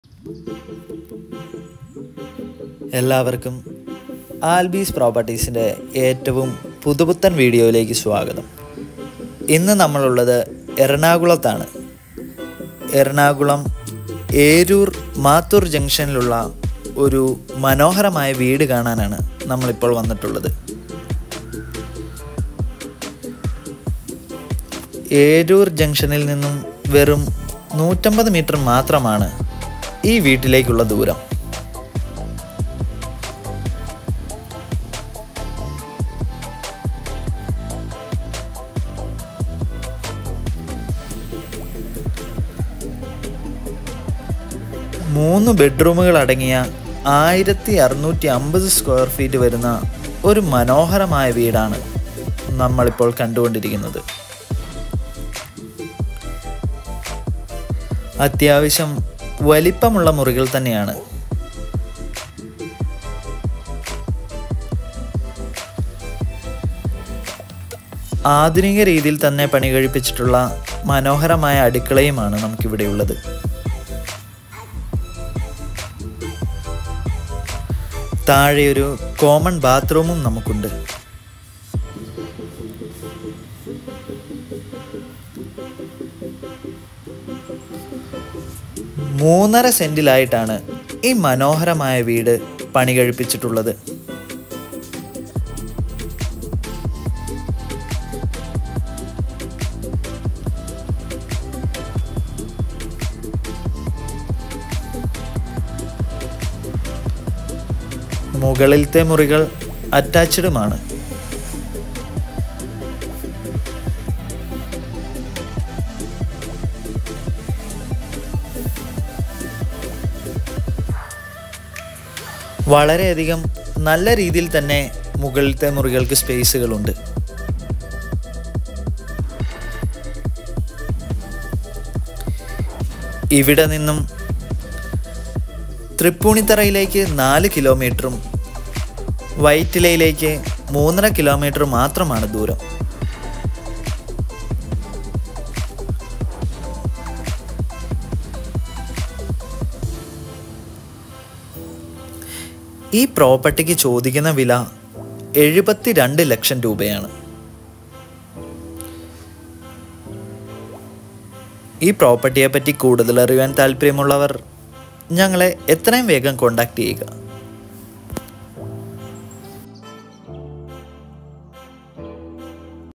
Beautiful 1650 Sqft House For Sale In Ernakulam
1650 sqft House

3 bhk

3.300 cent

Thrippunithura 4 km

Vytilla 3.5 km